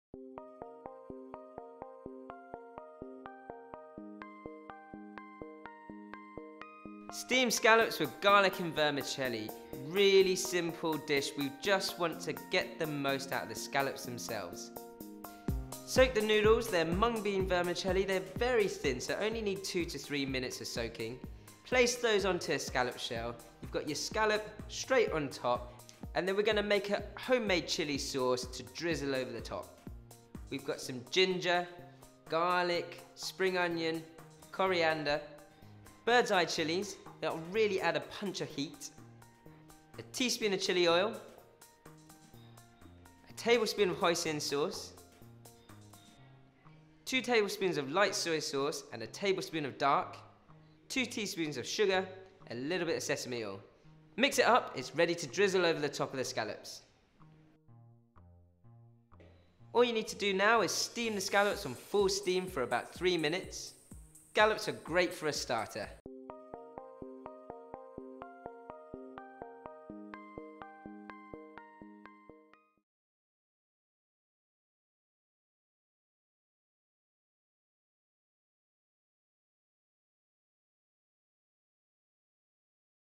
Jeremy Pang's School of Wok Steamed Scallops | Ideal World
Jeremy Pang shows how to cook scallops